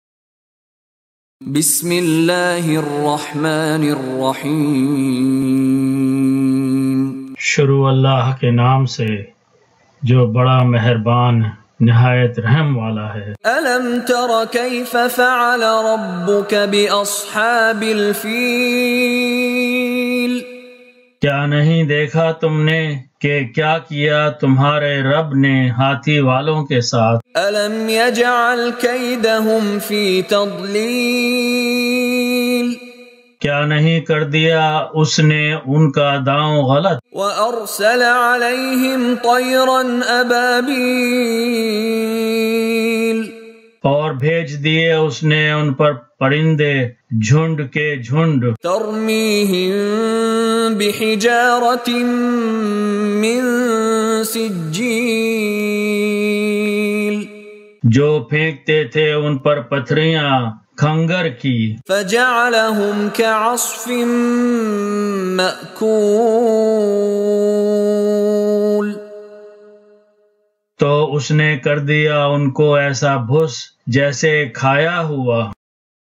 AlQuran Colors 105 AlFeel Arabic Urdu Text Audio
Surah 105 AlFeel
Learn Quranic Meanings with
Word by Word Colored Arabic Urdu
Colored Arabic Nastaleeq Text
Colored Urdu Text Hafiz Khan
Recitation Mishary AlAfasy
Urdu Narration Hafiz Khan